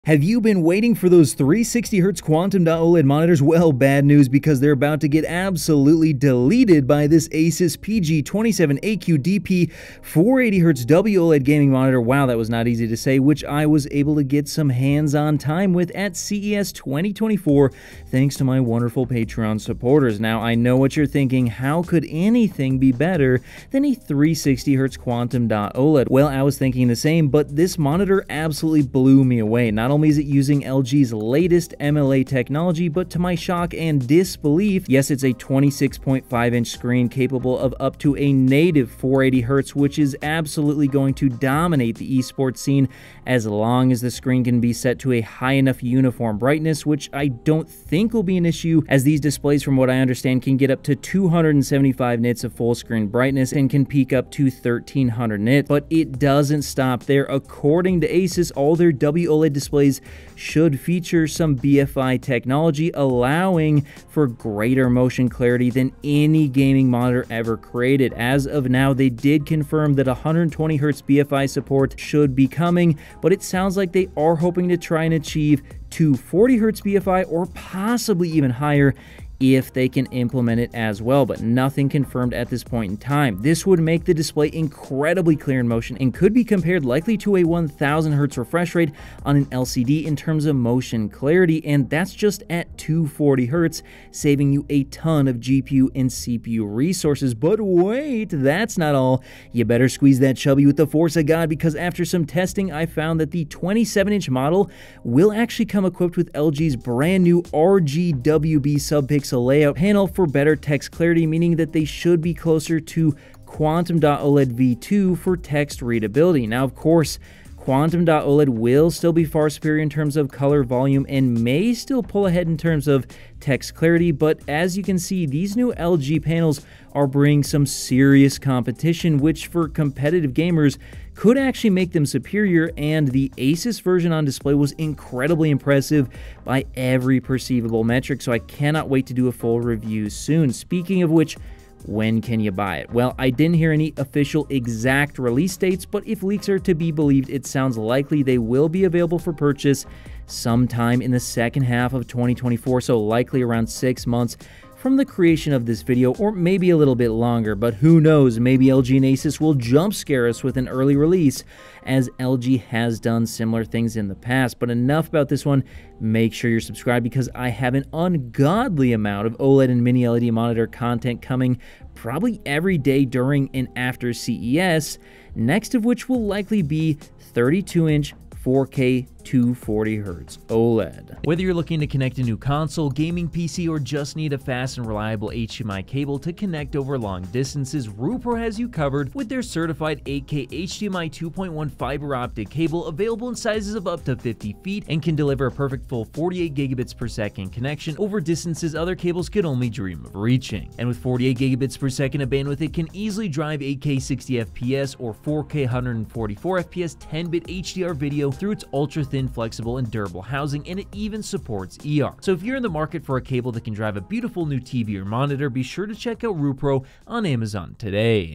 Goodbye 240Hz OLED - ASUS PG27AQDP 480Hz Monitor (CES 2024)
UPDATE!!!

I went back to the ASUS room and upon closer inspection something looked a bit off about the 27” 480Hz.

According to TFT the clear protective film had not been removed 😂😂😂

It will be matte 🤮

Funny that at arms length even just leaving the protective film on looks much better.

Have you been waiting for those 360Hz QD OLED monitors, well bad news, because they’re about to get absolutely deleted by this ASUS PG27AQDP 480Hz WOLED gaming monitor which I was able to get some hands on time with at CES 2024 thanks to my Patreon supporters.

Now I know what you're thinking, how could anything be better than a 360Hz QD OLED, well I was thinking the same, but this monitor absolutely blew me away. Not only is it using LGs latest MLA technology, but to my shock and disbelief its using one of the clearest glossy finishes I’ve ever seen, and after talking to ASUS representatives they confirmed that yes, this will be the final coating which buyers should expect, which is a very nice change to see from the usual hideous matte finish LG usually violates their WOLED monitors with. In fact this coating was so clear that despite rocking a 1440p resolution it actually looked far sharper and more colorful than even a 4K monitor they had on display close by.

Now as for the rest of the specs, yes it is a 26.5” screen capable of up to a native 480Hz, which will absolutely dominate the E-sports scene as long as the screen can be set to a high enough uniform brightness, which I don’t think will be an issue as these displays from what I understand can get up to 275nits of full screen brightness and can peak up to 1,300 nits.

So not only is the display able to pump out an additional 120Hz over the QD OLED alternative, but it should also be up to 30% brighter AND the glossy coating is significantly better than the gloss available on the QD OLEDs in my opinion as well, which unfortunately suffer from a magenta tint under even the smallest exposure to ambient light.

But it doesn’t stop there. According to ASUS all their WOLED displays should feature some sort of BFI technology allowing for even greater motion clarity than any gaming monitor ever created. As of now they did confirm 120Hz BFI support, but it sounds like they are hoping to try and achieve 240Hz BFI or possibly even higher if they can implement it well.

This would make the display incredibly clear in motion and could be compared likely to a 1000Hz refresh rate on an LCD in terms of motion clarity.

BUT WAIT that’s not all, after some testing I found that the 27” model will actually come equipped with LGs new RGWB subpixel layout panels for better text clarity meaning that they should be closer to QD OLED V2 for text readability.

Now of course QD OLED will still be far superior in terms of color volume, and may still pull ahead for text clarity, but as you can see these new LG panels are bringing some serious competition which for competitive gamers could actually make them superior, and the ASUS version on display was incredibly impressive by every perceivable metric, so I can’t wait to do a full review soon.

Speaking of which, when can you buy this?

Well, I didn’t hear any official exact release dates, but if leaks are to be believed it sounds likely they will be available for purchase sometime in the second half of 2024, so likely 6 months from the creation of this video or longer, but who knows, maybe LG and ASUS will jump scare us with an early release, as LG has done similar things in the past.

But enough about this one, make sure you're subscribed, because I have an ungodly amount of OLED and Mini LED monitor content coming probably every day during and after CES, next of which will likely be 32” 4K 240Hz OLED.